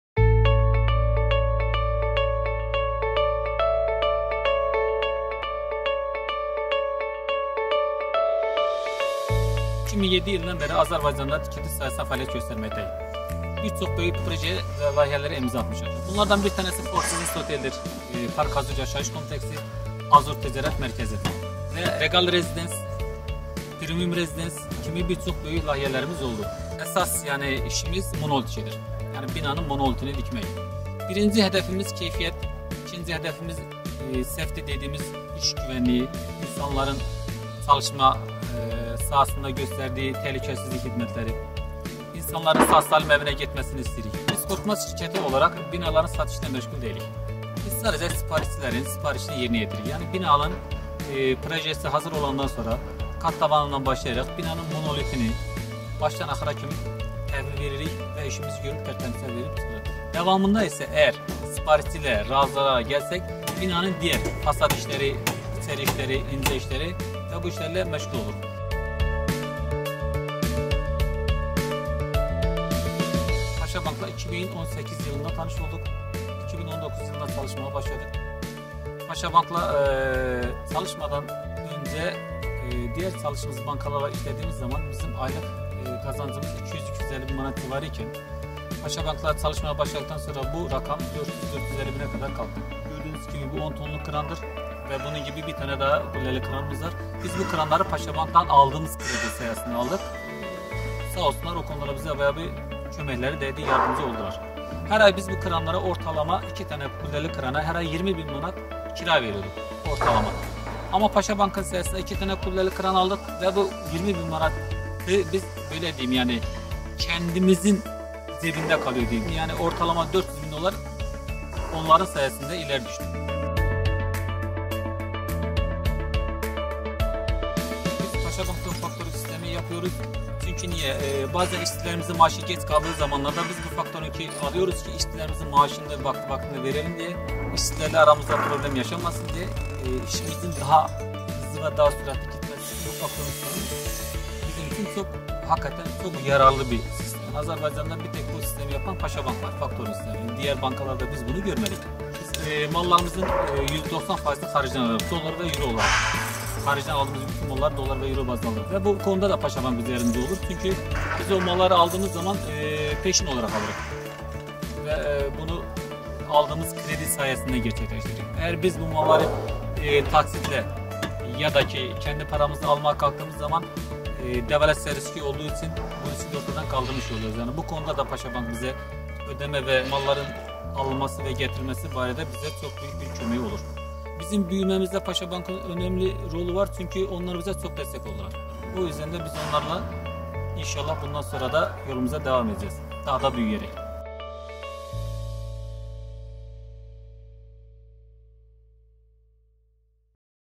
Biznes Klass: Korkmaz Group
Azərbaycanda 2007-ci ildən tikinti sahəsində fəaliyyət göstərən “Korkmaz Group” şirkəti bir çox uğurlu layihələrə imza atmışdır. “PAŞA Bank”la əməkdaşlığa başladıqdan sonra Bankdan əldə edilən kreditlər, faktorinq və digər məhsullar sayəsində həm şirkətin fəaliyyətini genişləndirmək, həm də xərcləri optimallaşdırmaq mümkün olmuşdur. Ətraflı məlumat “Korkmaz Group” şirkətinin təsisçisi və direktoru Mustafa Korkmazın müsahibəsində.

Buraxılış “PAŞA Bank”ın rəsmi səhifələrində 2021-ci ilin iyun ayında paylaşılıb.